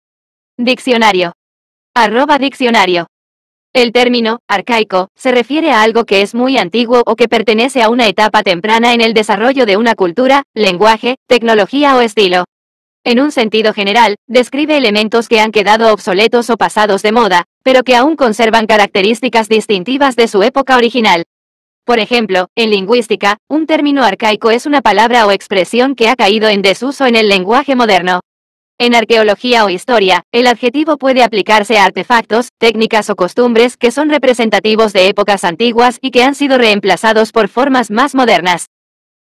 Arcaico Significado En Español
El término "arcaico" se refiere a algo que es muy antiguo o que pertenece a una etapa temprana en el desarrollo de una cultura, lenguaje, tecnología o estilo. En un sentido general, describe elementos que han quedado obsoletos o pasados de moda, pero que aún conservan características distintivas de su época original. Por ejemplo, en lingüística, un término arcaico es una palabra o expresión que ha caído en desuso en el lenguaje moderno. En arqueología o historia, el adjetivo puede aplicarse a artefactos, técnicas o costumbres que son representativos de épocas antiguas y que han sido reemplazados por formas más modernas.